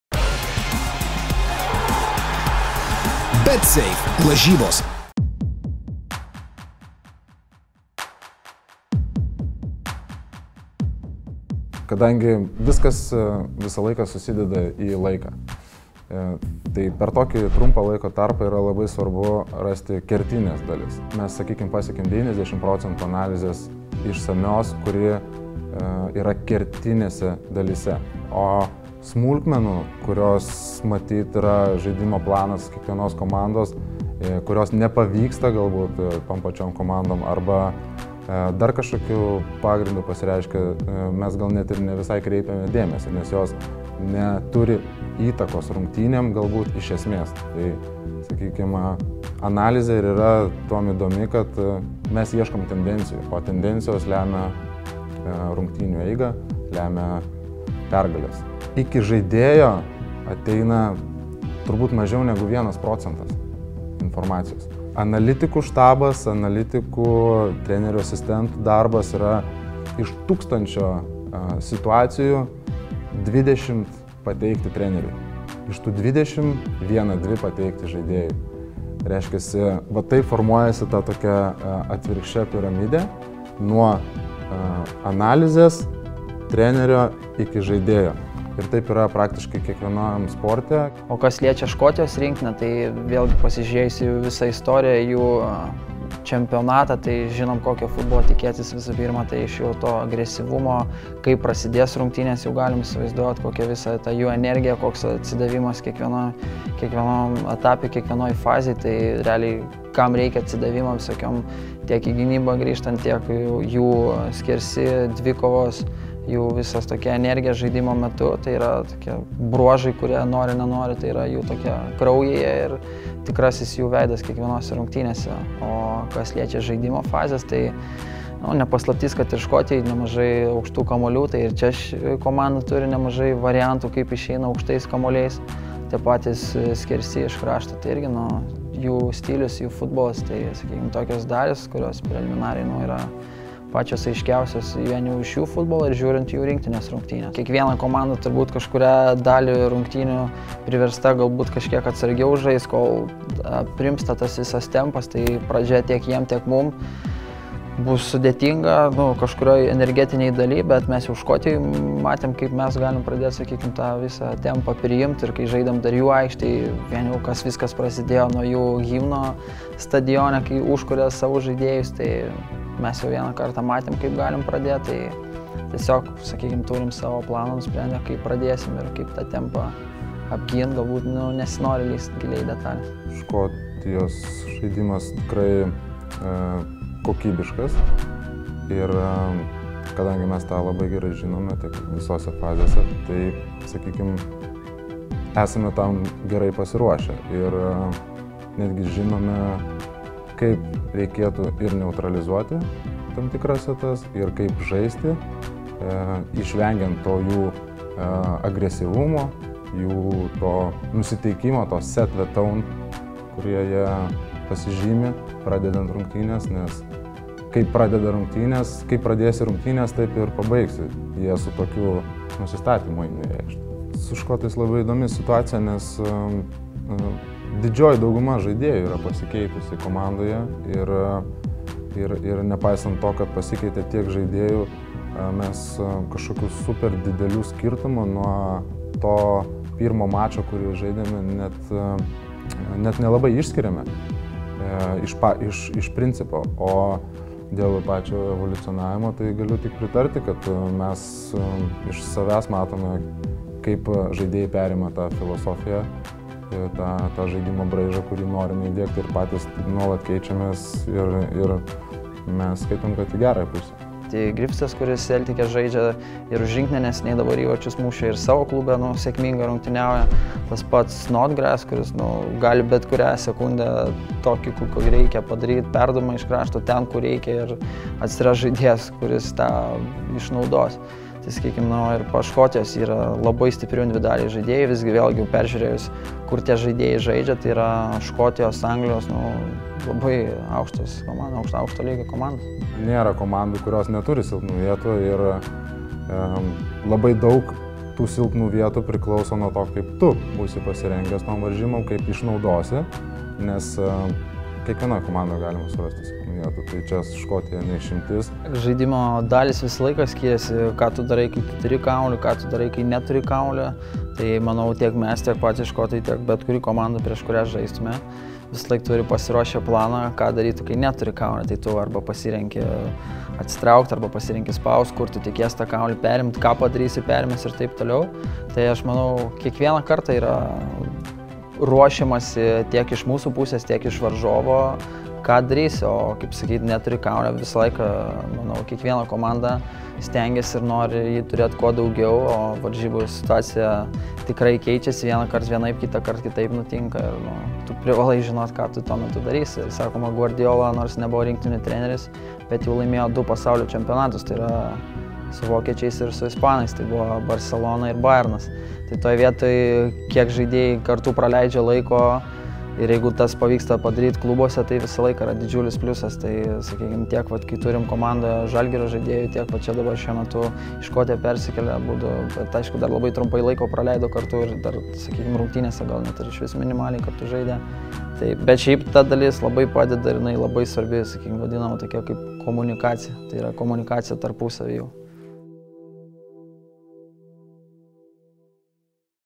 Trenerių analizė: žaidėjus pasiekia vos 1% informacijos
Lietuvos rinktinės analitikai Marius Babravičius ir Rokas Garastas peržiūri šimtus valandų vaizdo medžiagos ir renka kiekvieną smulkmeną apie mūsų varžovus.

„Mes ieškome tendencijų, o tendencijos lemia rungtynių eigą ir pergales. Analitikų štabo darbas iš 1000 situacijų apie 20 pateikti treneriui, o iš tų 20 tik 1-2 perteikti žaidėjui“, - sakė specialistai.

Išsamiame vaizdo reportaže škotų rinktinės stipriosios pusės, varžovų lyderiai, silpnosios vietos ir analitikų darbo specifika!